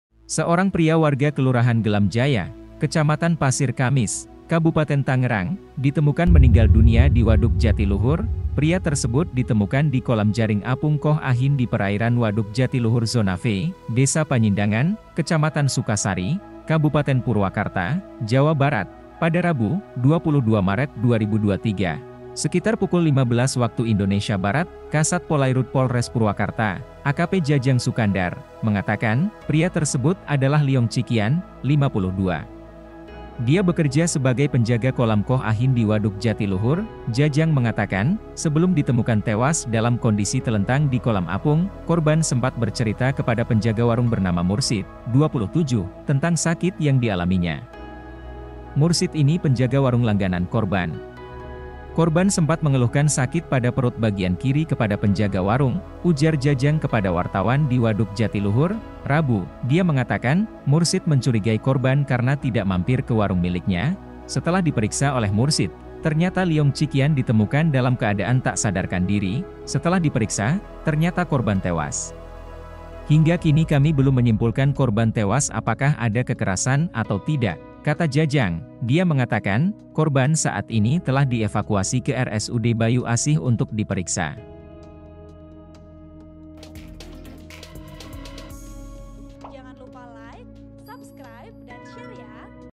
Penjaga Kolam Asal Tangerang Tewas di Waduk Jatiluhur Purwakarta, Sempat Keluhkan Sakit Perut
Berita selengkapnya, klik tautan berikut ini:
Penulis: Deanza Falevi | Editor: Giri |Video Editor: Uben